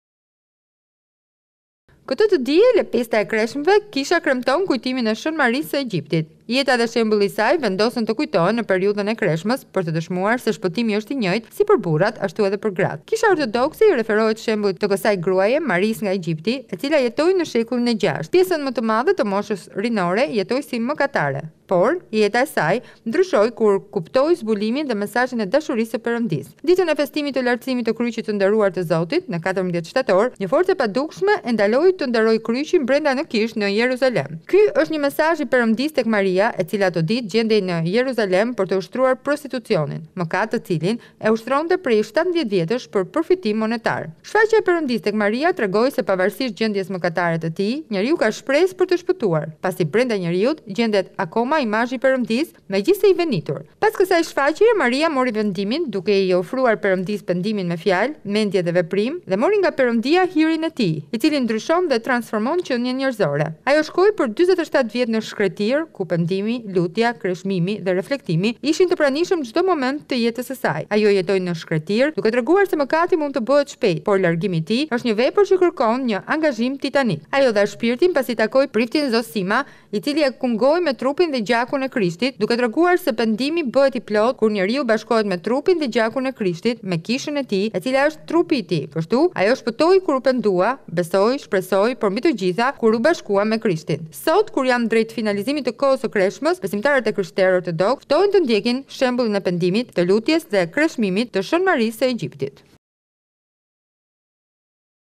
E Diela e Pestë e Kreshmës, besimtarët të ndjekin shembullin e pendimit, lutjes dhe kreshmimit
Durrës    E Diela e Pestë e Kreshmës, besimtarët të ndjekin shembullin e pendimit, lutjes dhe kreshmimit